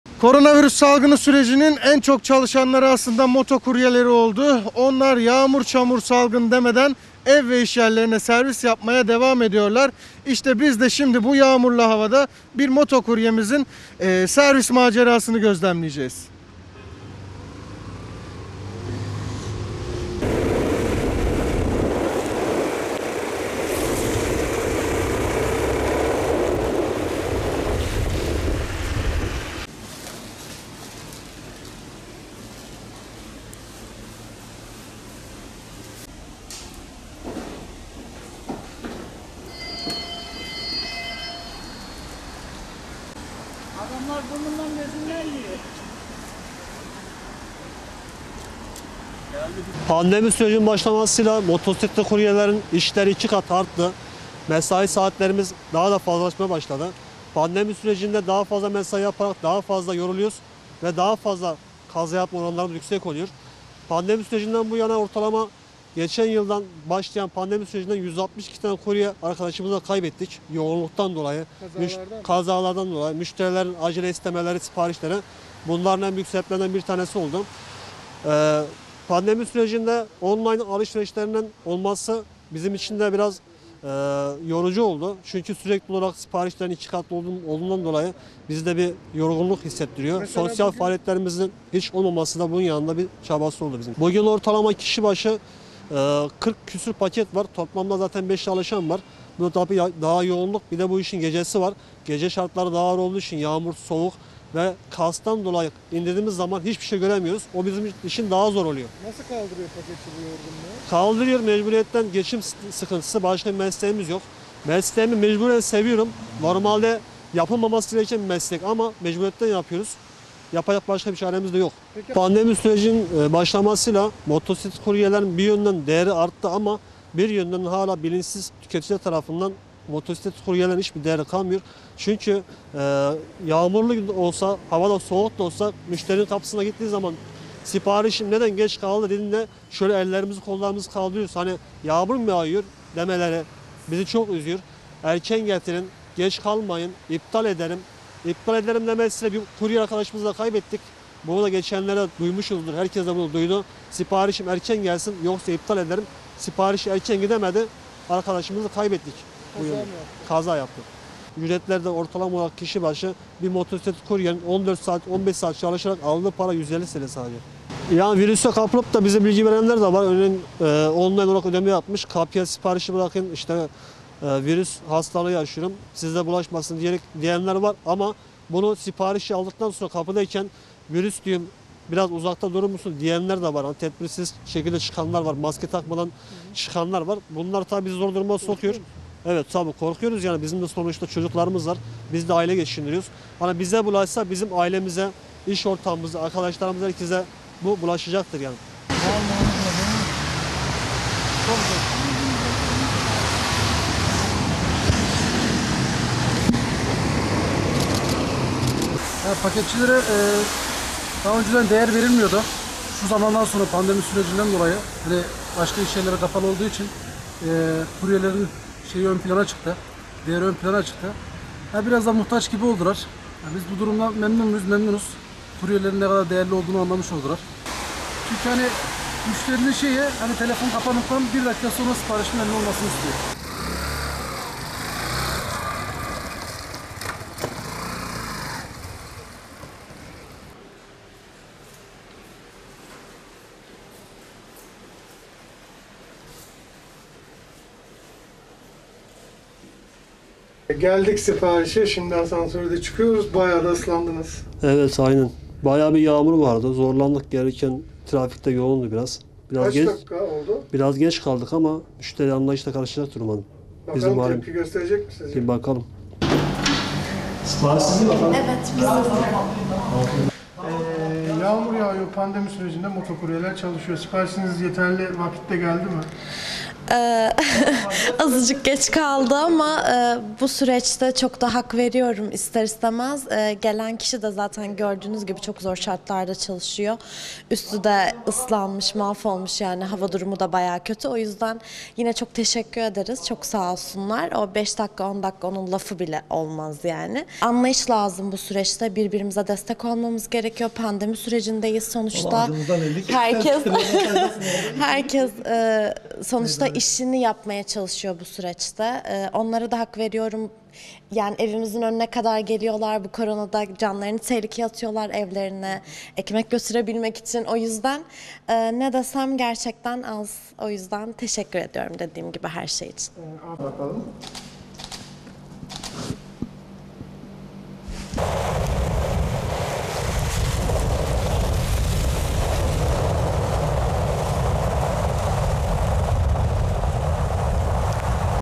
Pandemide iş yüküyle birlikte artan kazalarda 160'tan fazla kurye hayatını kaybetti
Koronavirüs salgınının dünya genelinde etkisini göstermesiyle birçok iş sektörü durma noktasına gelirken, motosikletli kuryeler ise en yoğun çalıştıkları dönemi yaşıyorlar. Günde yaklaşık 14 saat çalıştıklarını vurgulayan Adana Motosikletli Kuryeler Derneği Başkanı Yalçın Parmak, martta pandemi sürecinin başlamasından itibaren yoğunluktan ve yorgunluktan dolayı 160'tan fazla motosikletli kuryenin çalışırken kaza sonucu hayatını kaybettiğini söyledi.

Dünya genelinde koronavirüs salgının etkisini artırmasından sonra birçok kişi evlerinden çalışmaya başlarken, tedbirler kapsamında restoran ve kafeler de paket siparişle çalışmalarını sürdürdü. Yiyecek- içecek sektörünün yani sıra giyim ve teknoloji gibi sektörlerde de alışverişlerin büyük çoğunluğu internet üzerinden yapılmaya başlandı. Salgın şartları birçok mesleğin yavaşlamasına ya da durmasına yol açarken, internet ve telefon üzerinden siparişle alışverişin bu yoğunluğu motosikletli kuryelerin mesaisini 14 saate kadar çıkardı. Sokağa çıkma kısıtlamasının olduğu günler de dahil yoğun bir mesaiyle çalışan her sektörden motosikletli kuryeler, özellikle kış günlerinde yağmurlu ve soğuk havalarda zorlu şartlara rağmen işlerini yürütmeye çalışıyor. İş yükü artan kuryelerin en büyük şikayeti ise 5- 10 dakika için müşterilerden aldıkları tepkiler oldu. Pandemi sürecinde saatlerce çalıştıklarını ve sosyal hayatlarının kalmadığını dile getiren Adana Motosikletli Kuryeler Derneği Başkanı Yalçın Parmak, koronavirüs salgının başlamasından itibaren yoğunluktan ve yorgunluktan dolayı 160'tan fazla motosikletli kuryenin çalışırken kaza sonucu hayatını kaybettiğini söyledi.

'GEÇ KALDIN, İPTAL EDİYORUM GİBİ TEPKİLER BİZİ ÜZÜYOR'

12 yıldır motosikletli kurye olarak çalıştığını ve bir süre sonra arkadaşlarıyla birlikte dernek kurduklarını belirten Yalçın Parmak, çalışma şartlarını iyileştirmek için ellerinden geleni yaptıklarını söyledi. Koronavirüs salgını sonrası iş yoğunluklarının iki katına çıktığını kaydeden Parmak, bununla birlikte yorgunluğa ve hızlı çalışmaya bağlı kazaların da arttığını vurguladı. Bunun en büyük nedeninin anlayışsız müşteriler olduğunu söyleyen Parmak, "Online alışveriş hız kazanınca biz tamamen devreye girdik. Çalıştığım iş yerinde toplamda 5 motosikletli kurye var. Her birimiz bugün 40’ın üzerinde pakete çıktık. Hala da devam ediyor. Bu süreçte bir kesim değerimizi anlasa da bazı müşteriler hala anlayışsız davranıyor. Yağmur, çamur demeden götürdüğümüz bir siparişin karşılığında ‘Neden geç kaldın, iptal ediyorum’ gibi tepkiler almak bizi çok üzüyor. Günlük 150 TL'ye saatlerce çalışan insanlarız. Müşterilerin bazıları telefonu kapatır kapatmaz siparişleri gelsin istiyor. Bizim sektörde anlayış diye birşey yok" dedi.

'5- 10 DAKİKANIN LAFI OLMAZ'

Yalçın Parmak’ın çalıştığı restorandan yemek sipariş eden ve teslim alan Özge Yıldız ise siparişinin biraz gecikmeli geldiğini ama bu süreçte bunu sorun etmenin anlamsız olduğunu söyledi. Kuryelerin ellerinden geleni yaptığını ifade eden Yıldız, "Gelen arkadaşın üstü başı ıslanmış, mahvolmuş. Böyle bir durumda 5- 10 dakikanın lafı dahi olmaz. Her şey için teşekkür etmek gerekir. Anlayışlı olmayan birçok insan var. Unutmamak gerekir ki bu insanlar biz evde otururken zor şartlarda hizmetlerini sürdürüyorlar" diye konuştu.